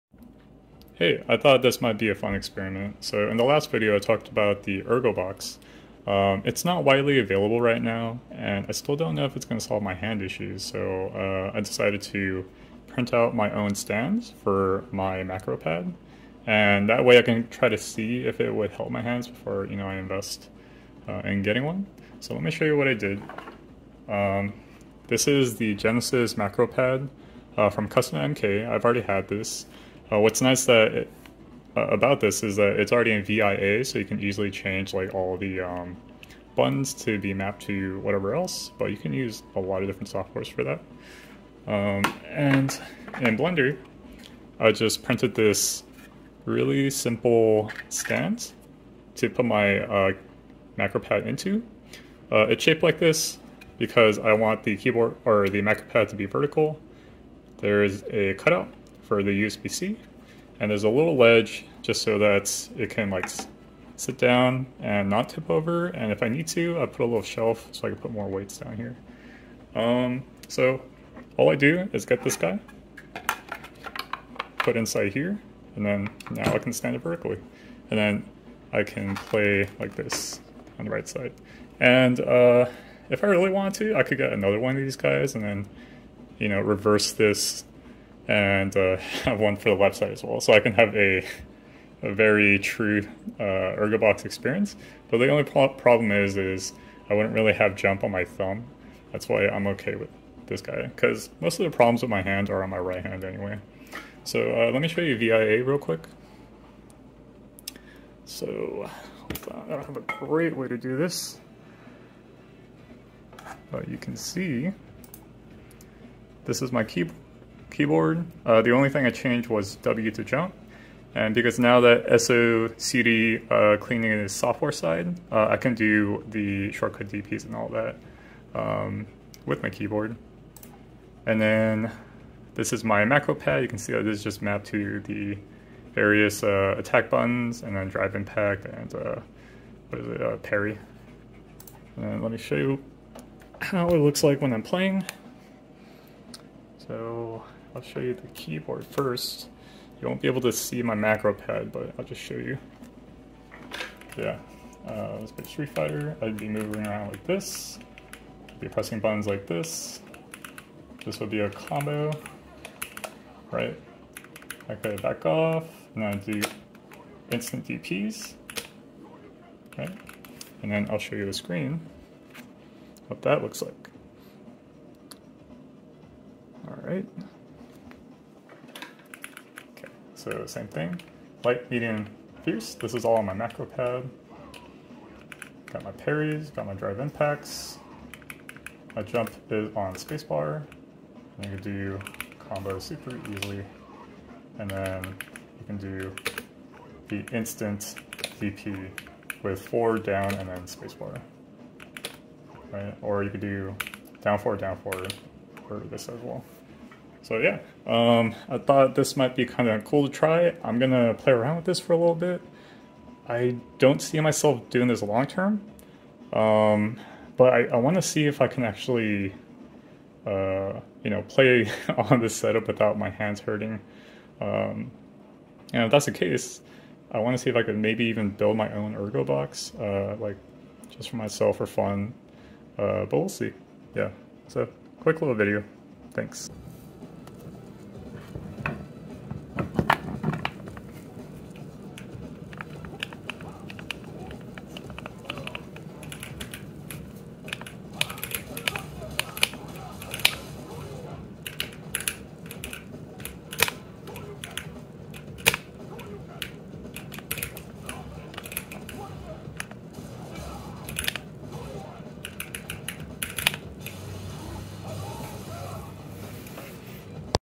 Ergonomic Hitbox Experiment - Keyboard/Macropad
This is a test to see how I can turn my PC setup into something more ergonomic for fighting games. Setup uses my keyboard, macropad, and a 3D Printed shelf.

Please check out ErgoArcade. They are the makes of the orignal Ergobox that made me want to attempt this experiment:

Timestamps:
0:00 Intro
1:50 VIA Setup
2:27 Usage
3:58 Macropad View